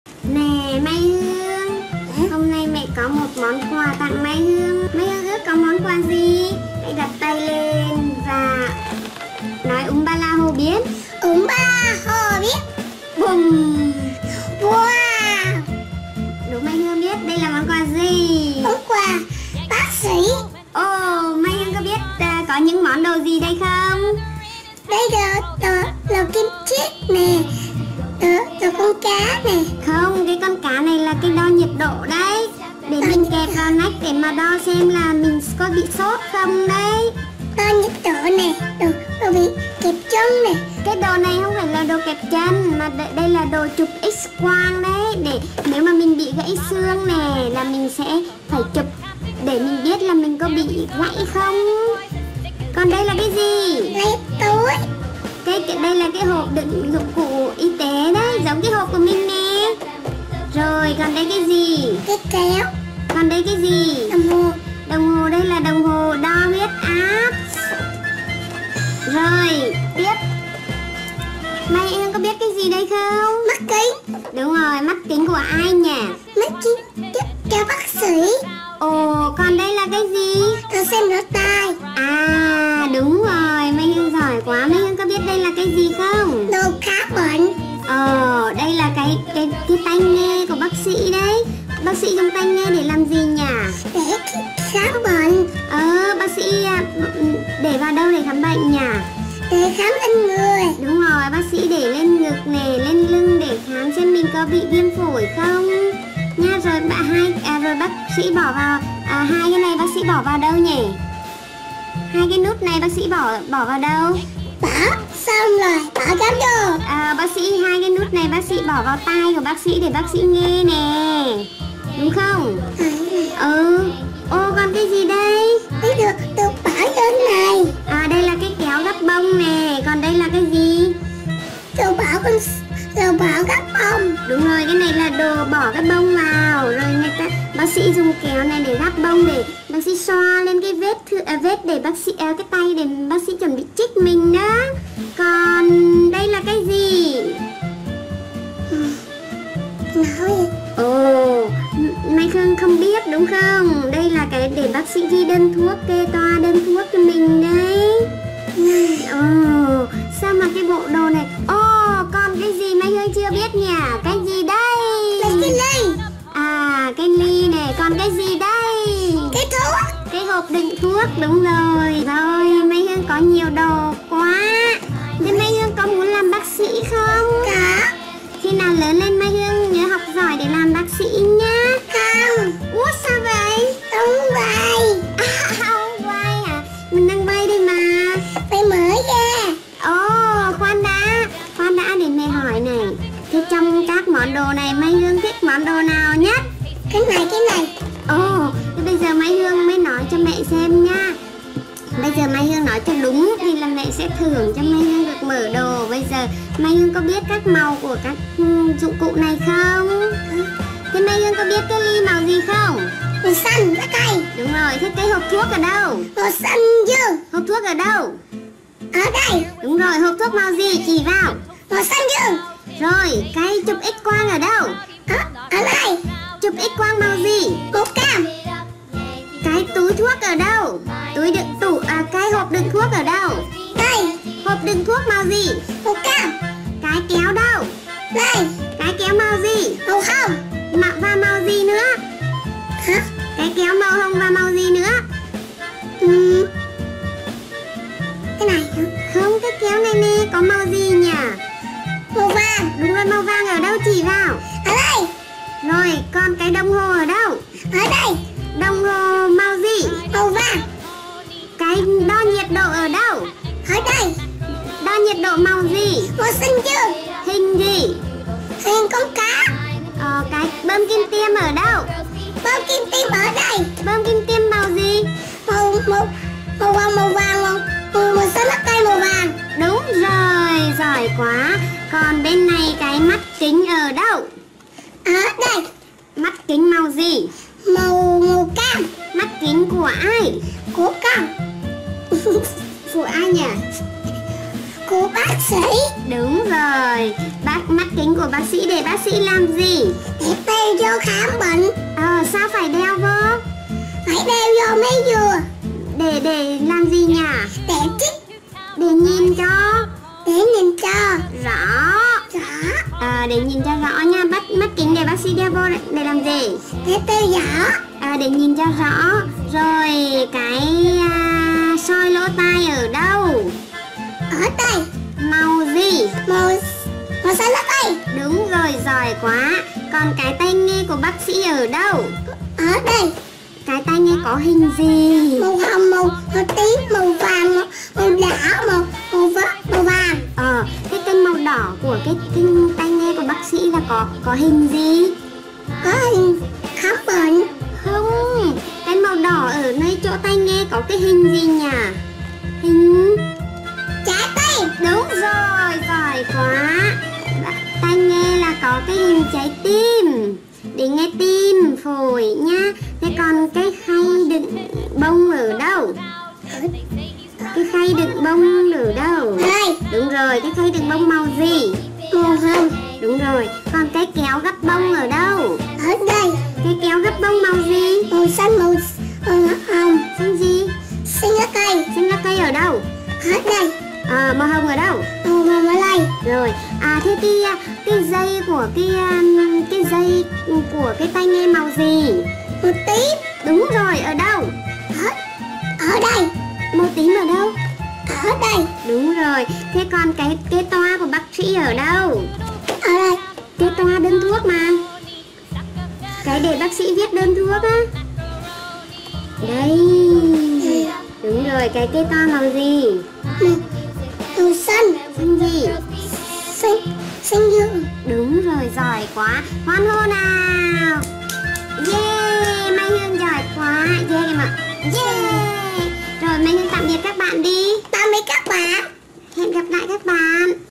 Review giới thiệu bộ đồ chơi bác sĩ khám bệnh và dạy bé tên các dụng cụ y tế, PA Channel
Hàng đẹp giá tốt ở đây: 1.  Đồ chơi bác sĩ HJ034A Shenzhen (giá 79k)
2. Bộ đồ chơi bác sĩ Doctor 8410D-1 (Giá: 310,000)
3. Bộ đồ chơi bác sĩ Hello Kitty Sanrio (Giá: 550,000)